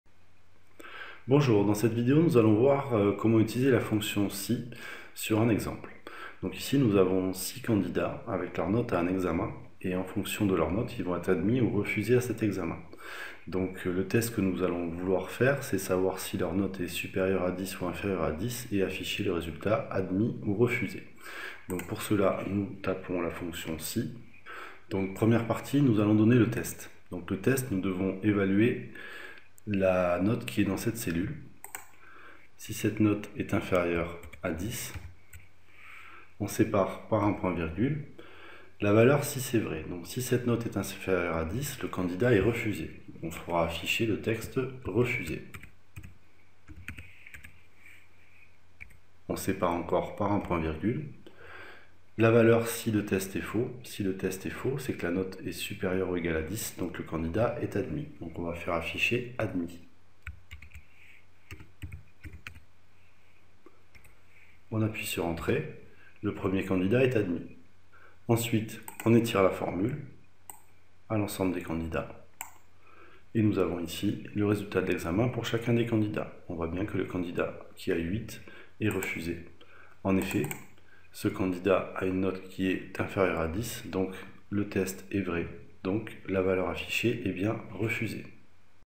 Tableur : utiliser la fonction SI
Tutoriel : apprendre à utiliser libreoffice et openoffice calc,les bases ( débutant )
Comment effectuer un test : vrai/faux sur la valeur d'une cellule avec la fonction SI donner une valeur conditionnelle.
La majorité des actions sont identiques ou très proches avec excel.